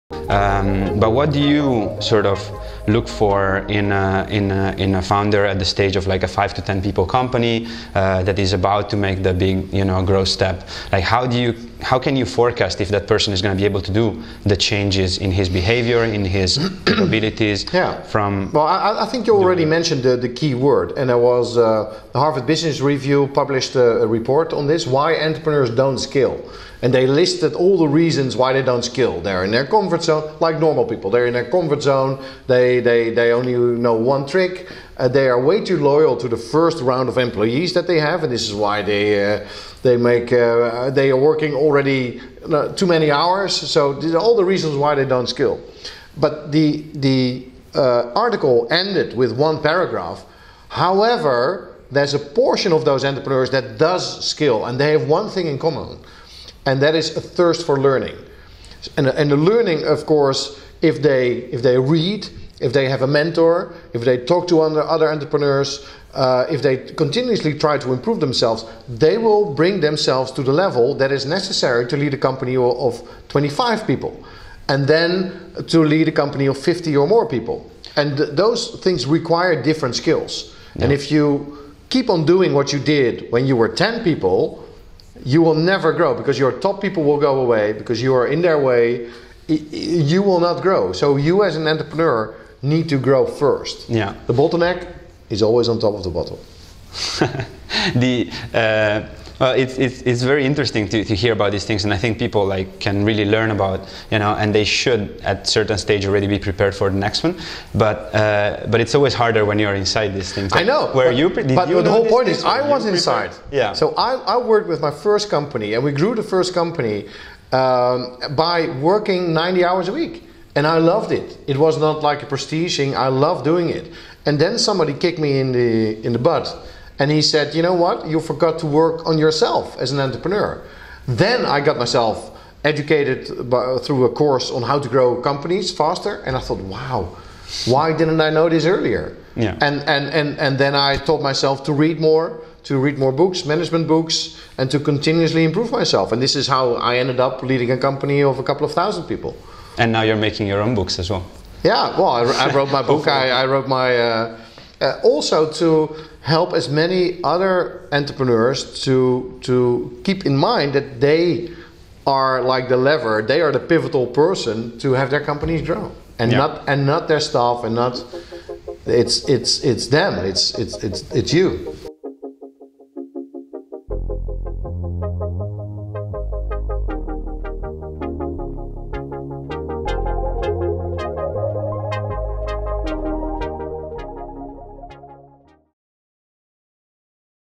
Scaling up your startup: founders must-haves [6/6] | Kees de Jong
In this insightful interview, Kees de Jong - a successful Dutch serial entrepreneur - shares his experience and advise on scaling up your startup and founders must have for success.

Curious about your company valuation?